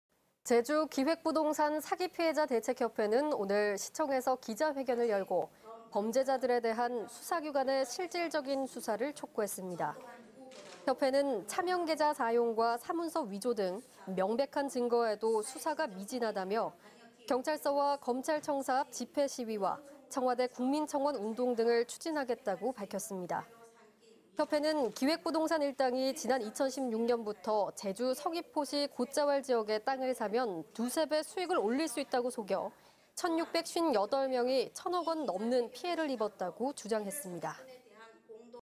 제주기획부동산 사기 피해자 수사 촉구 시위·국민청원 UBC프라임뉴스 20200720
제주기획부동산 사기 피해자 "수사 촉구..시위·국민청원"

 제주기획부동산 사기 피해자
대책협회는 오늘(20일)
시청에서 기자회견을 열고,
범죄자들에 대한 수사기관의
실질적인 수사를 촉구했습니다.

 협회는 "차명계좌 사용과
사문서위조 등
명백한 증거에도 수사가
미진하다"며 "경찰서와
검찰 청사 앞 집회 시위와
청와대 국민청원 운동 등을
추진하겠다"고 밝혔습니다.

 협회는 "기획부동산 일당이
지난 2016년부터 제주 서귀포시 곶자왈 지역의 땅을 사면
2~3배 수익을 올릴 수 있다고 속여 천658명이 천억 원 넘는
피해를 입었다"고 주장했습니다.@@